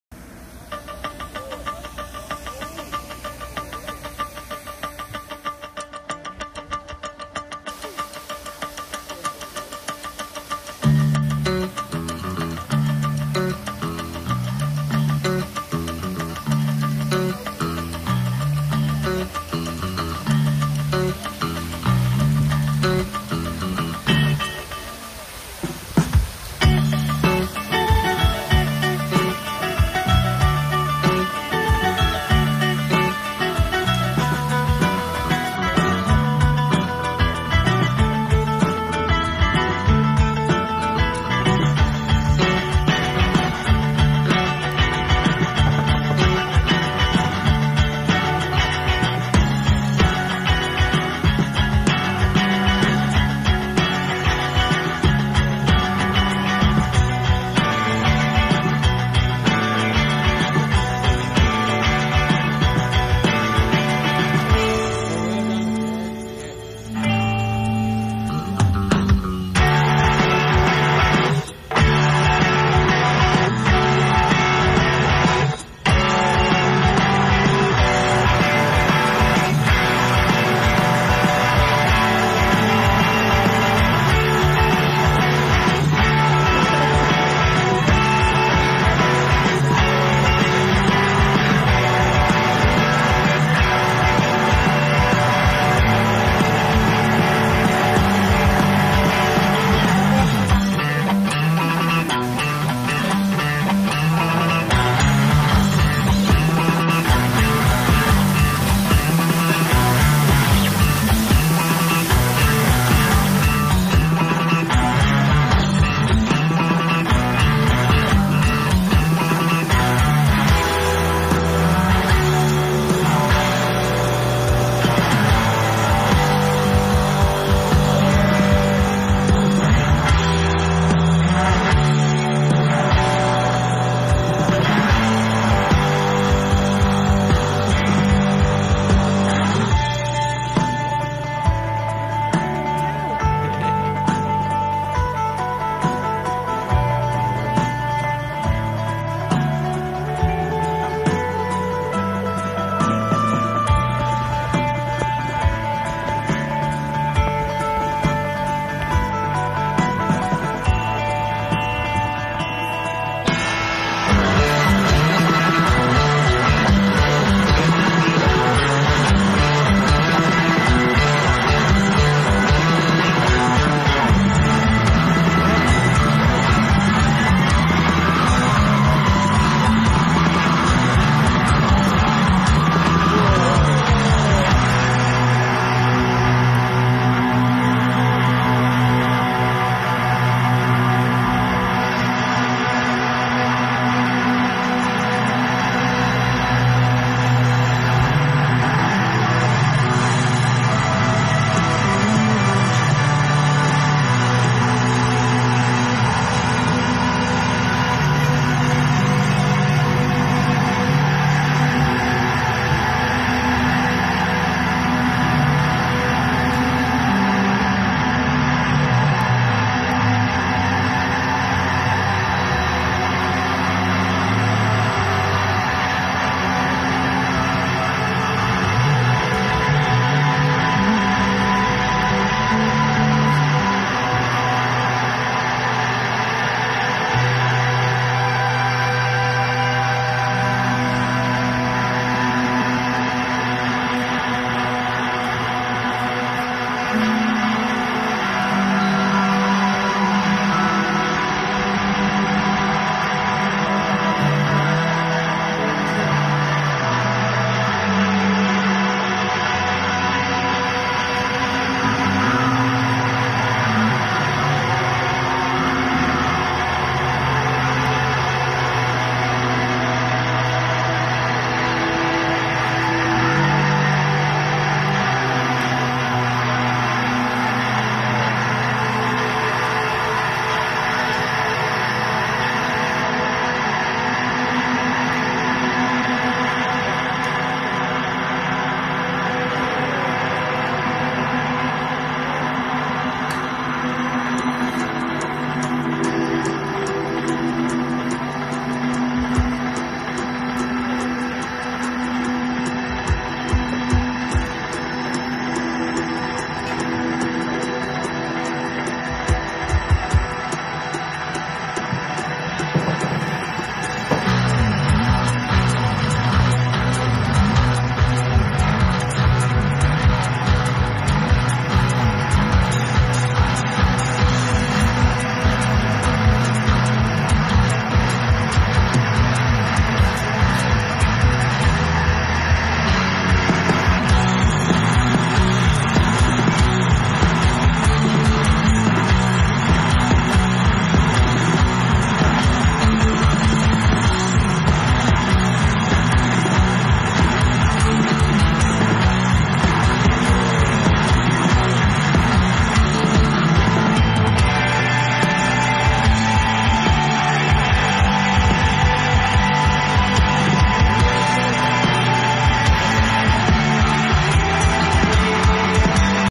Golfplex Family Golf Entertainment Centre Bear Mountain Adventure Golf Jungle & Polar Mini Golf New
Brand new Golfplex Near Bracknell Berkshire with driving range and the first 2 phases of adventure golf on the superb Bear Mountain Adventure Golf with the Jungle and Polar zones fully open and eagerly awaiting the Desert zone for 2022